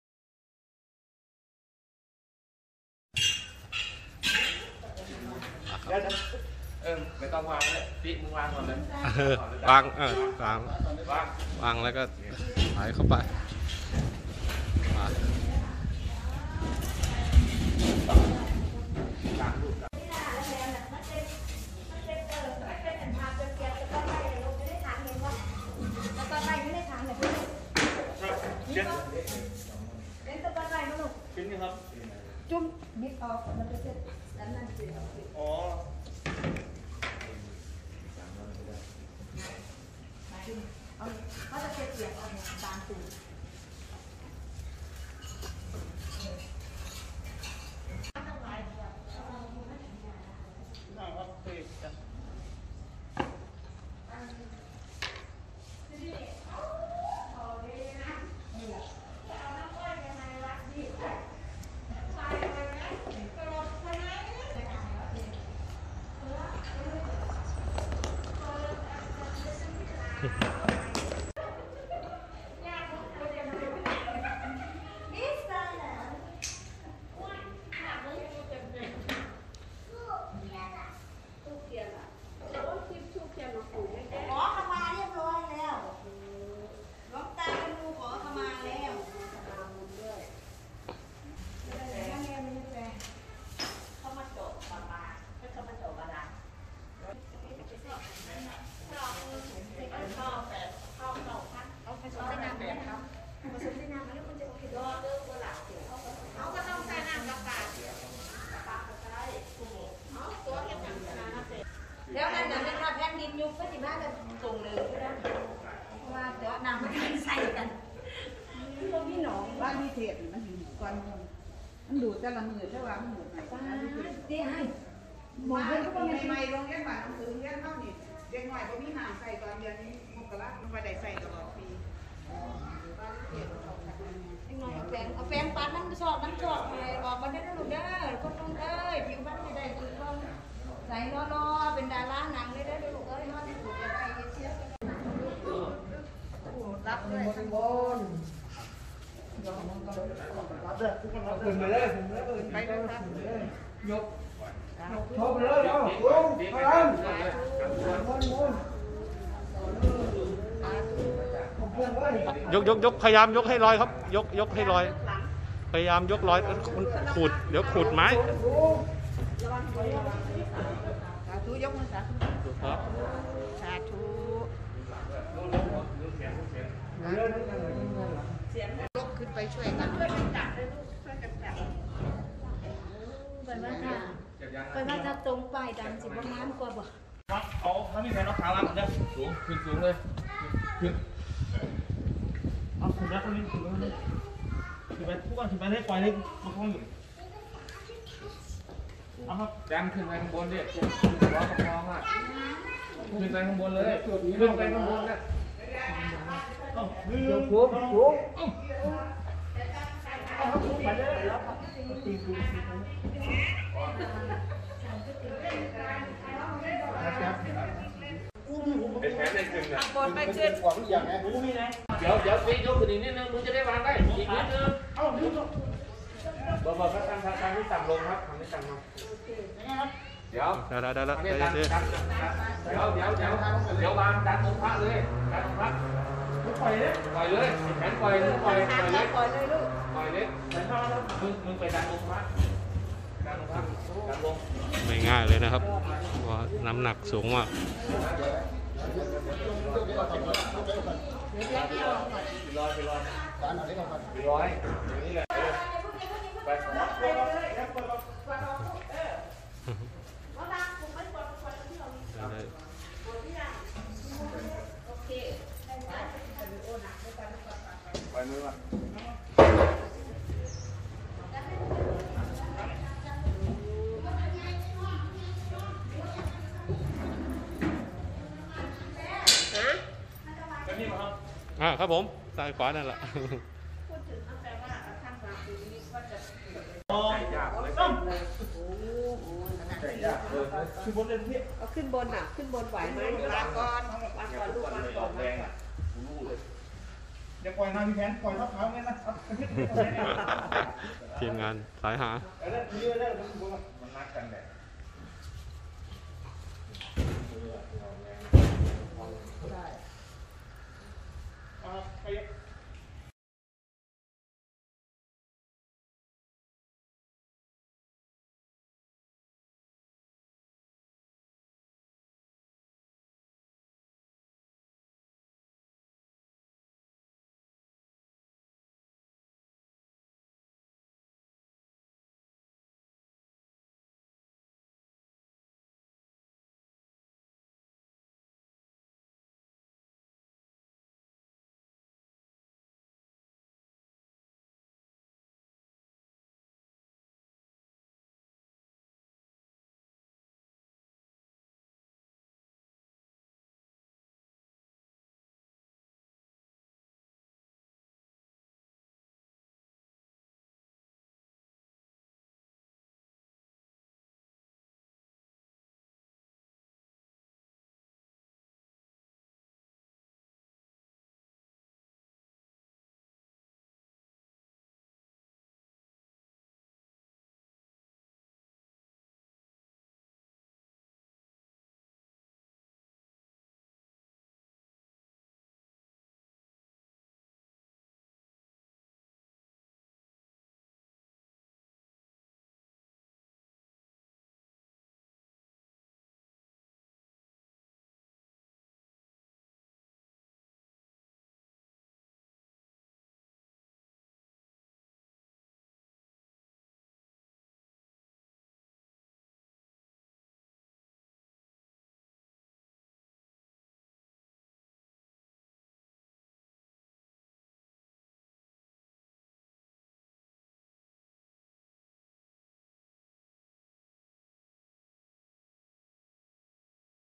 ถวายแท่นวางพระพุทธรูป
ถวายแท่นวางพระพุทธรูป งาน DIY ที่วัดประชานิมิตร อ.บัวใหญ่ จ.นครราชสีมา

รายละเอียดการสร้าง